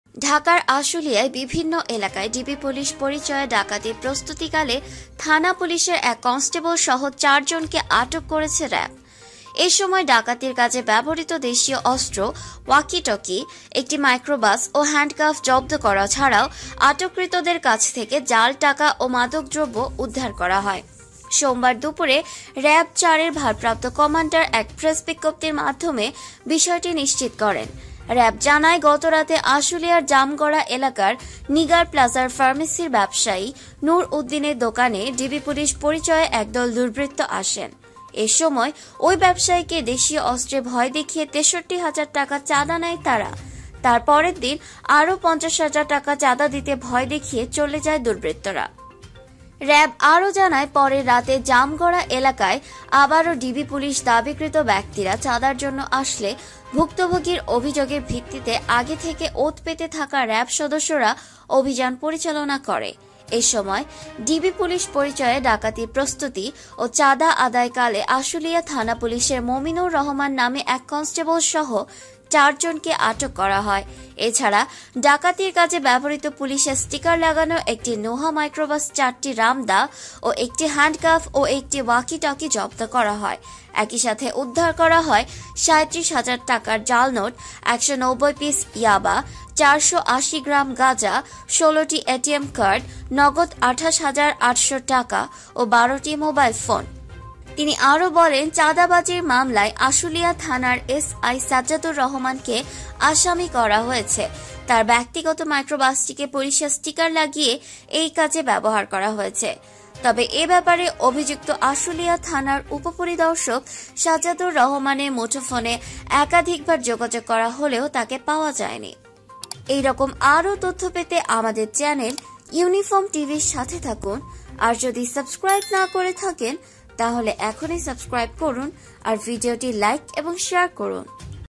শেষ রক্ষা হলনা পুলিশ মামার ডাকাতি করে ধরা পড়লেন তাও আবার র‍্যাবের হাতে। র‍্যাব ধরার পর কি মাইর দিলো
Youtube.com/c/UniformTV